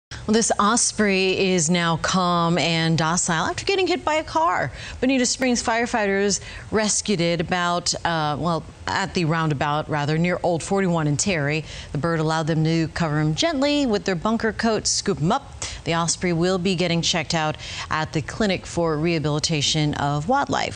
Osprey rescued by Bonita Fire
Bonita firefighters rescued an osprey that was hit by a car.